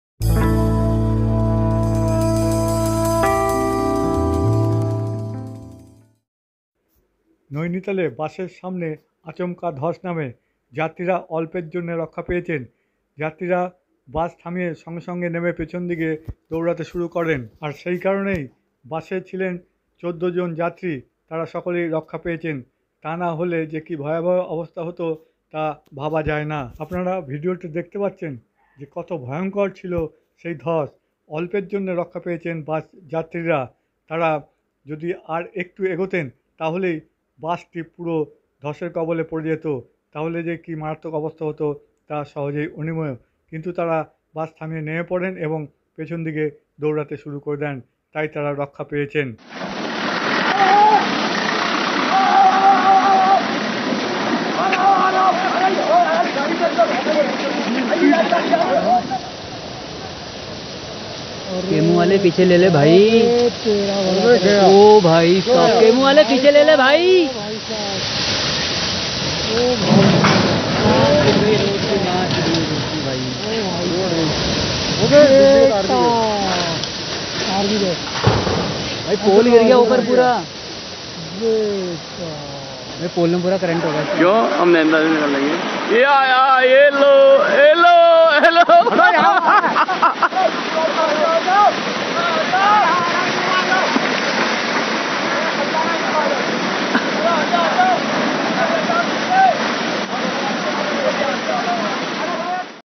নৈনিতালে বাসের সামনে হঠাৎই বিরাট ধস। তারপর কি হলো দেখুন। লাইভ।।
নৈনিতালে বাসের সামনে হঠাৎই ভয়াবহ ধস। তারপর কি হলো লাইভ দেখুন। বিস্তারিত জানতে ভিডিওটি দেখুন।